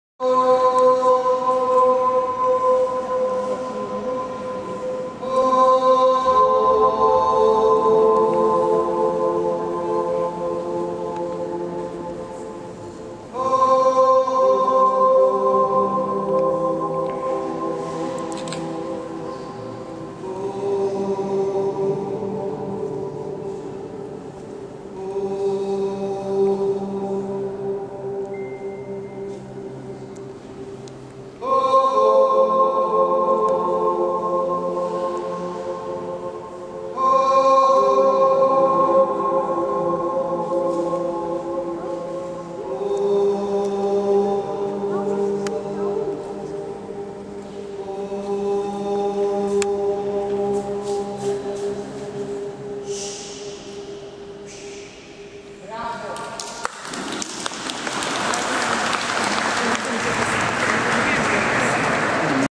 Echos in Battistero, Pisa, Italy
Every 10 minutes, a custodian demonstrates the unique echos within the Baptistry